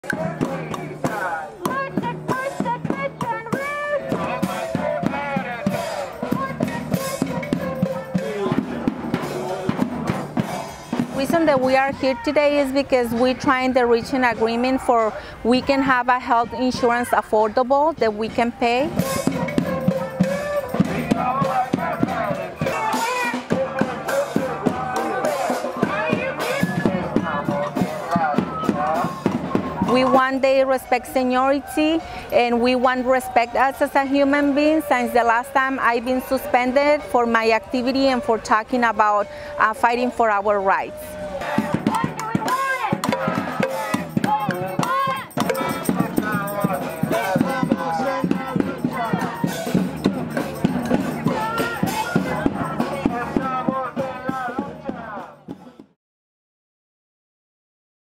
Employees at HS Lordships Restaurant protest for affordable health insurance
On Jan. 31, 2015, about 40 employees at Berkeley restaurant, HS Lordships, protested over health insurance cost.